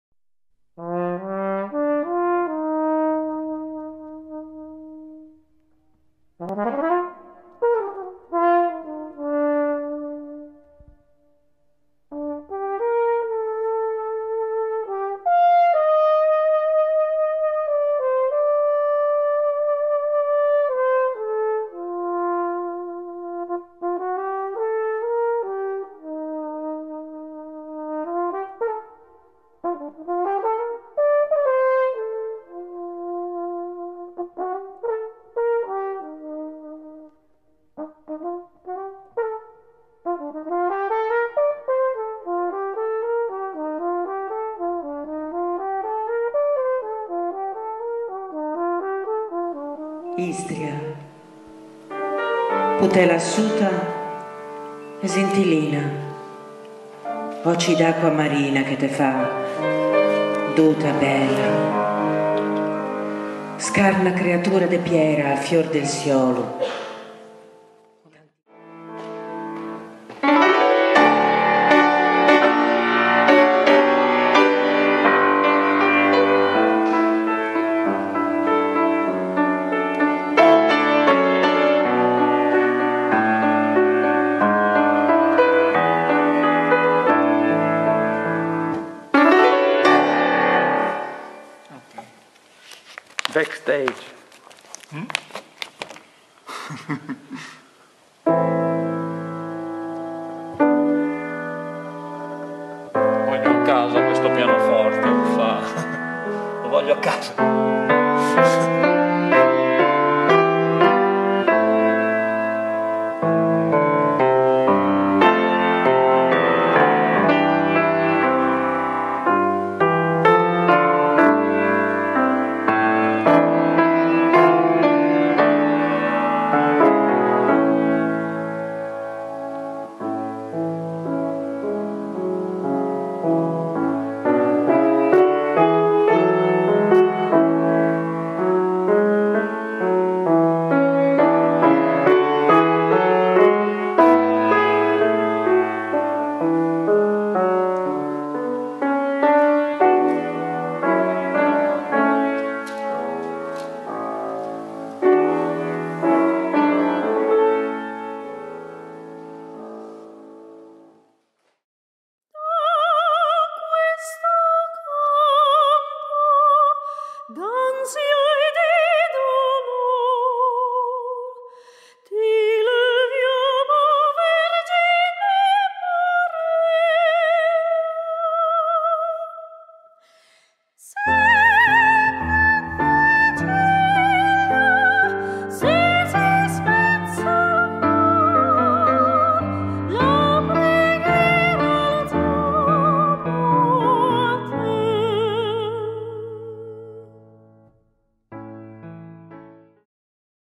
Dietro le quinte del Giorno del Ricordo
LA PRIMA PERFORMANCE ARTISTICA DEDICATA AL GIORNO DEL RICORDO

Un evento pluridisciplinare di musica e poesia.

Musica popolare dell'Istria veneta, canti dell'Esodo e una selezione di poesie di grandi poeti istroveneti del '900

Il gruppo, denominato MITTELEUROPA ENSEMBLE  (quartetto) è disponibile per la commemorazione del GIORNO DEL RICORDO proponendo una performance di musica popolare dell'Istria veneta, canti dell'esodo e letture di selezionate poesie di poeti istroveneti del '900.

La formazione del gruppo:

Sabrina Sparti, canto
Laura Bagarella, voce recitante
Mario Fragiacomo, tromba e flicorno
Roberto Favilla, pianoforte